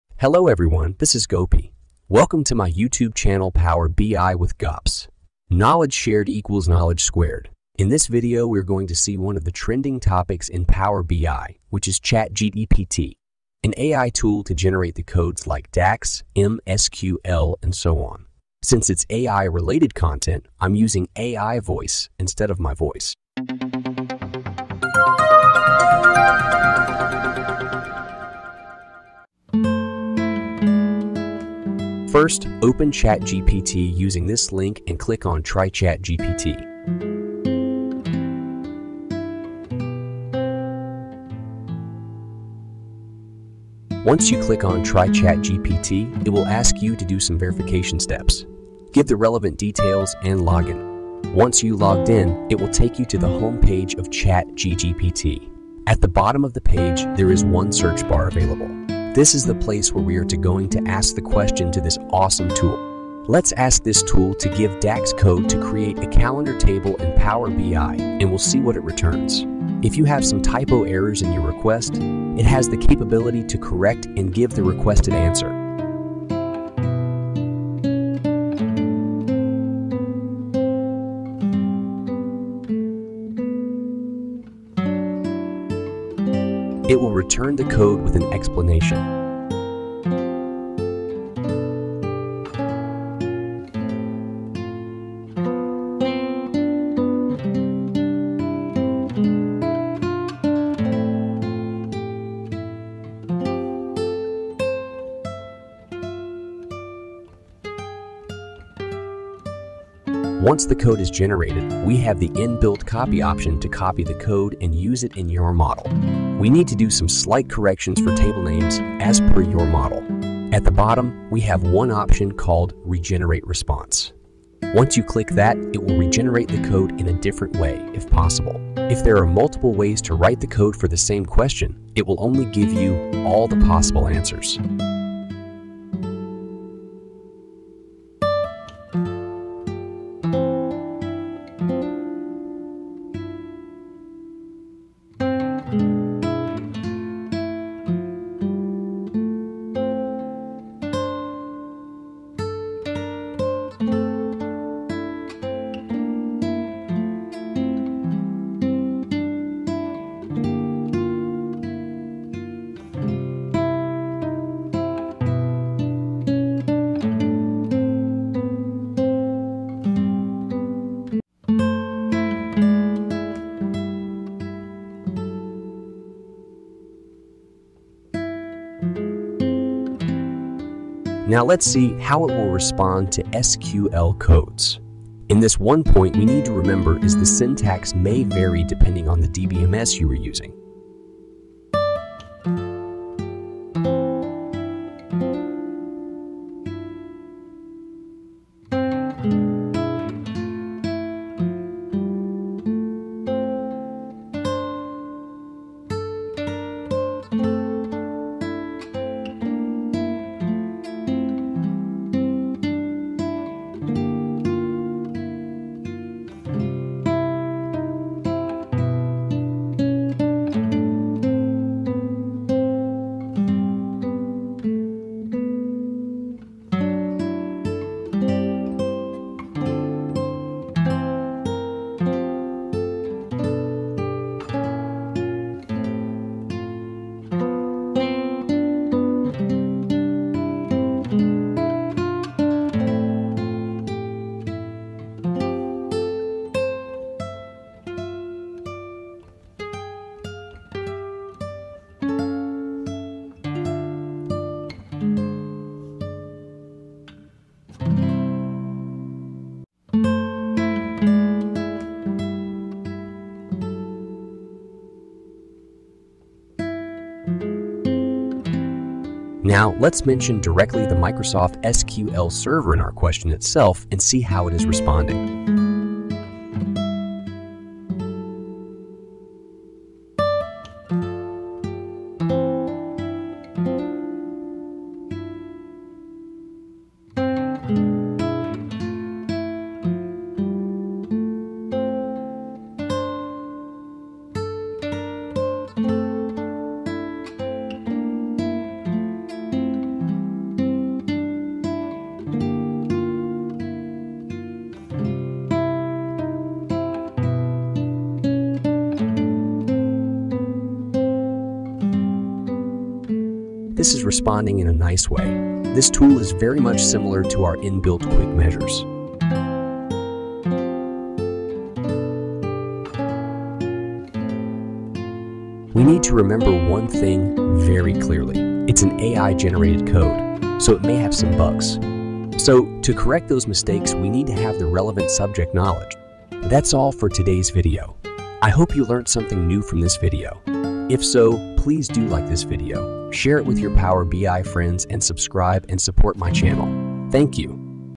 How to use ChatGPT - AI Tool to generate codes - Power BI with Gops | Demo Tutorial
In this video, we are going to see How to use ChatGPT - AI Tool to generate codes.